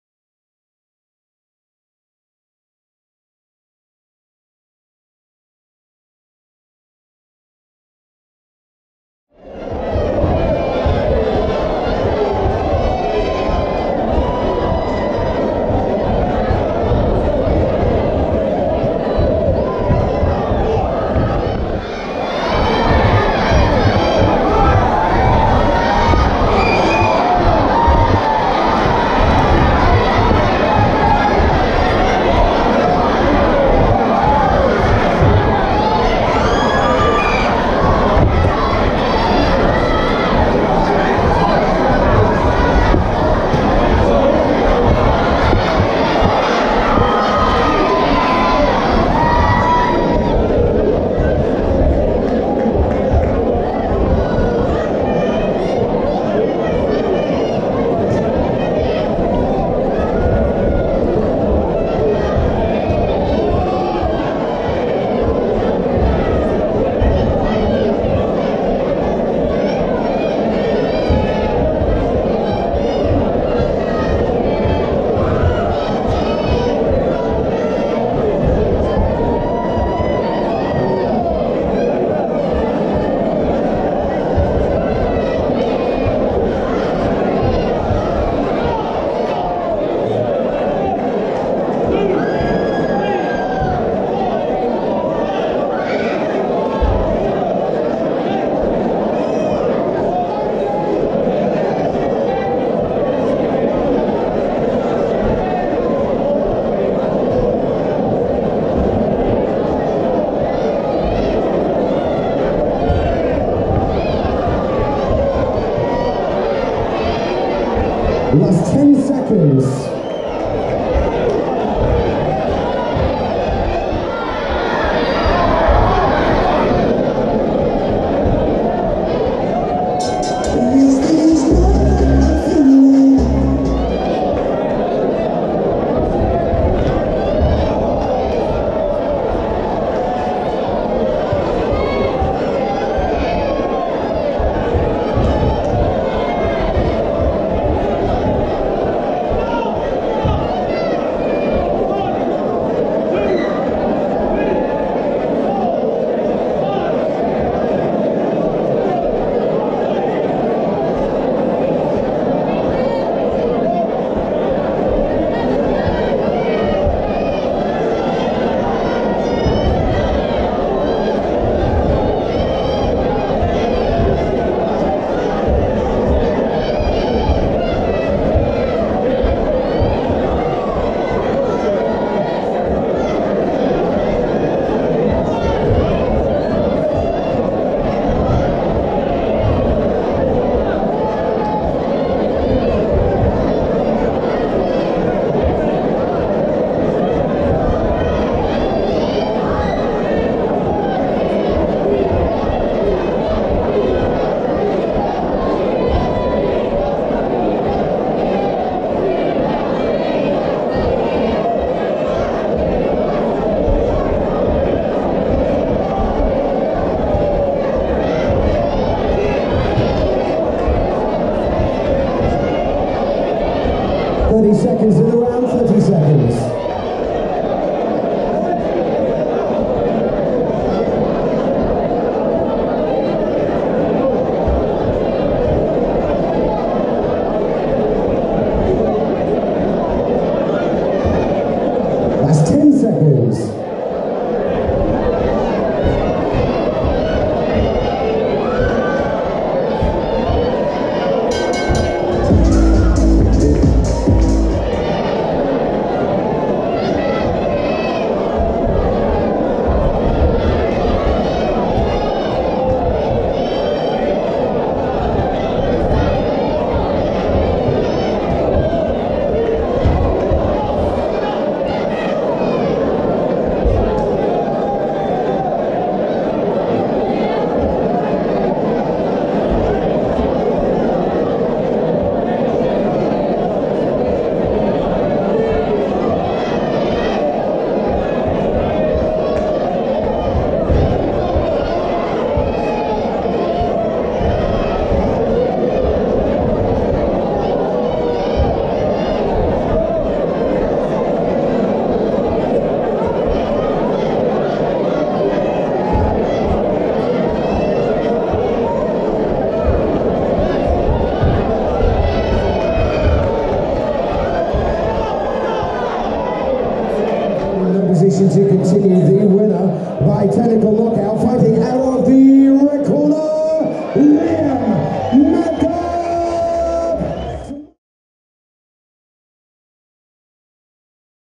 Ultra White Collar Boxing | Grimsby | Liam Smart VS Dean Moore
Grimsby Ultra White Collar Boxing 18th November 2017

8 Weeks FREE training / Get in the best shape of your life/ Raise money for Cancer Research UK